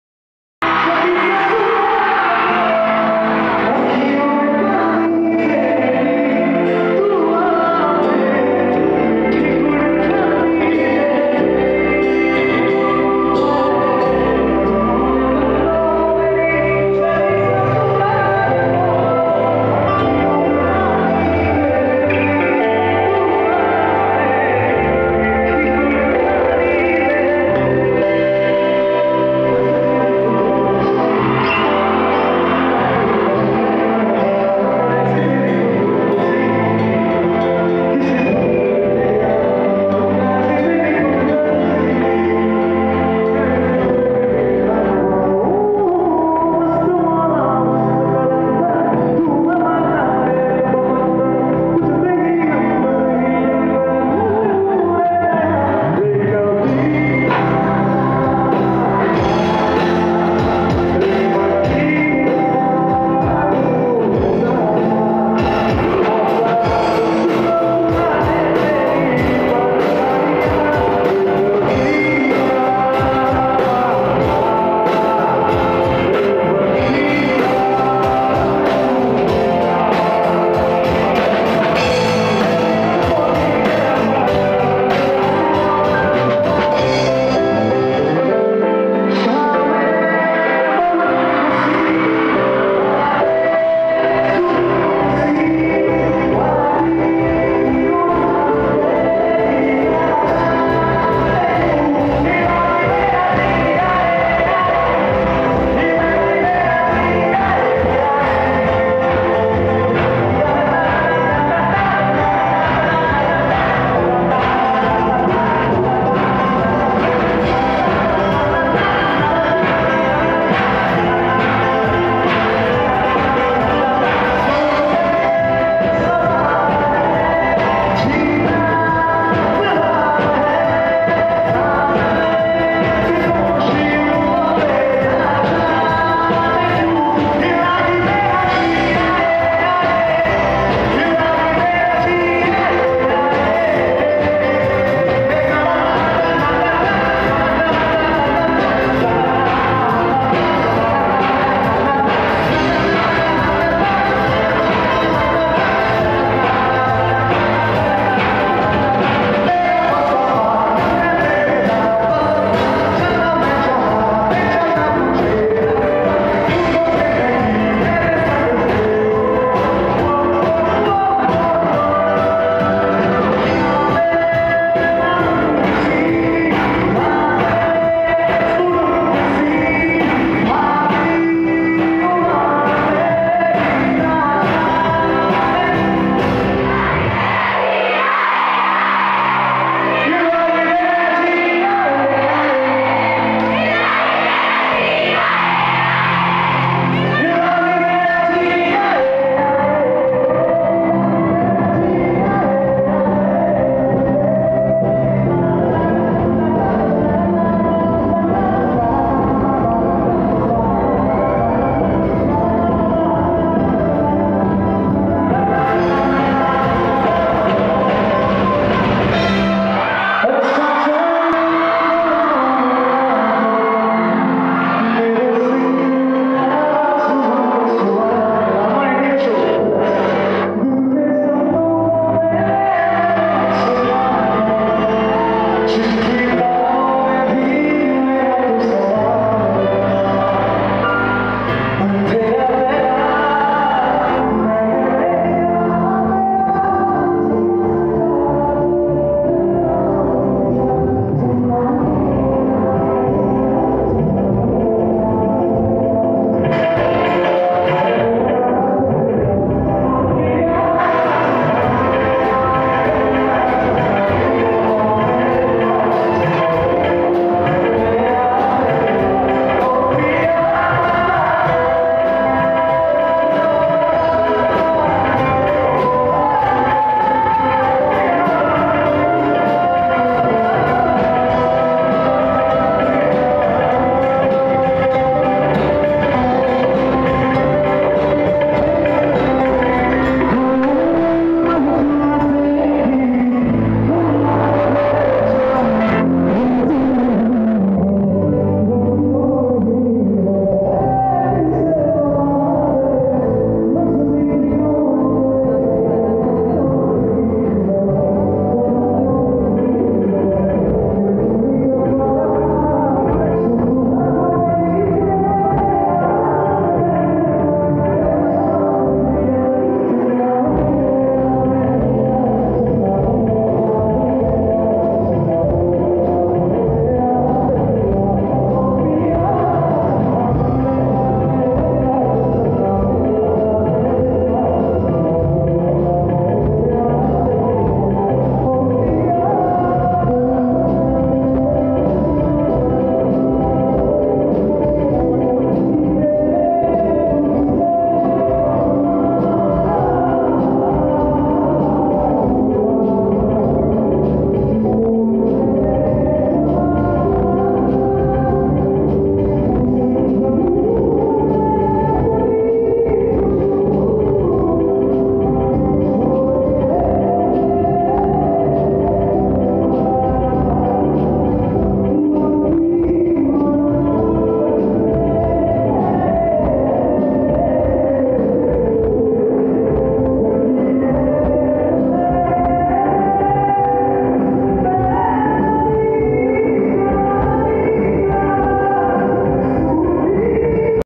RIK BASU Live... Booking & Information- 9830961650
Rik The Rockstar Live & Loud at Calcutta University Centenary hall.
a short video clip for those people who missed last night the performance of RIK in the city of joy KOLKATA